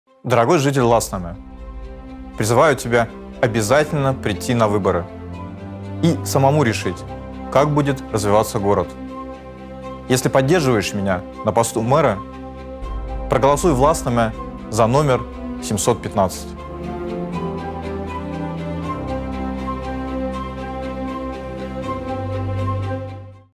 В Ласнамяэ голосуй за Михаила Кылварта, номер 715
Если поддерживаешь меня на посту мэра, проголосуй в Ласнамяэ за номер 715